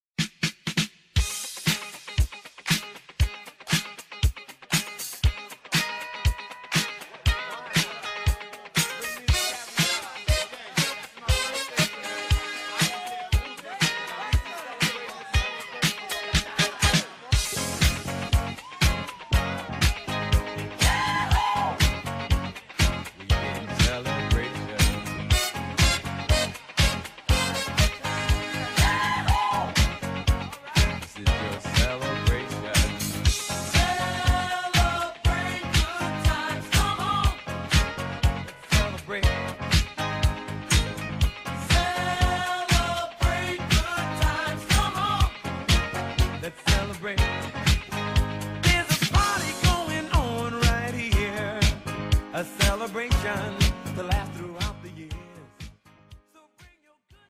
ITG 20th Anniversary
On June 17, 2022, the ITG Team held a sunset ceremony at the Holcom Premises (terrace) to honor 20 years of achievement, open access, and innovation.
We're excited to make the upcoming years even better and greater!